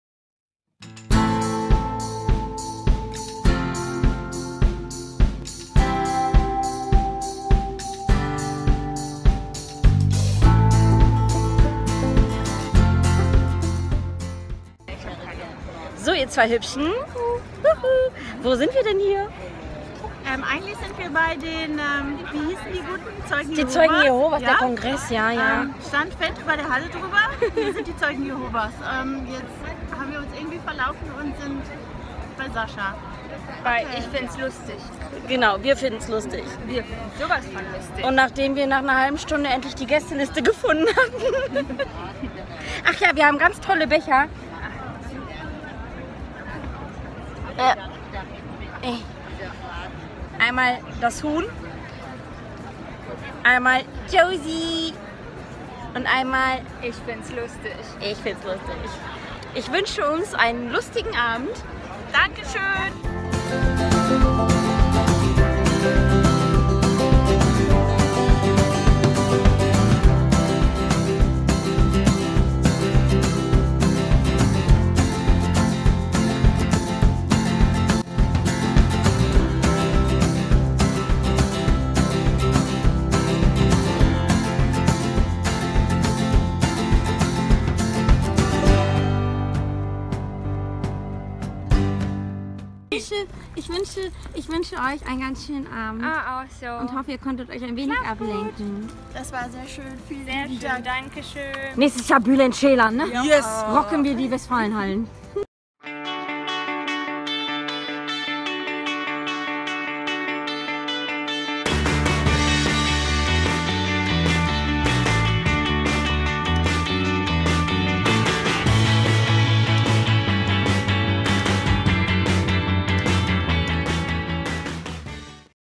Junge Erwachsene mit Krebs bei Sascha Grammel Dortmund
Einen Abend lang alles vergessen und einfach nur lachen. Das ermöglicht Euch "Wir können Helden sein!"e.V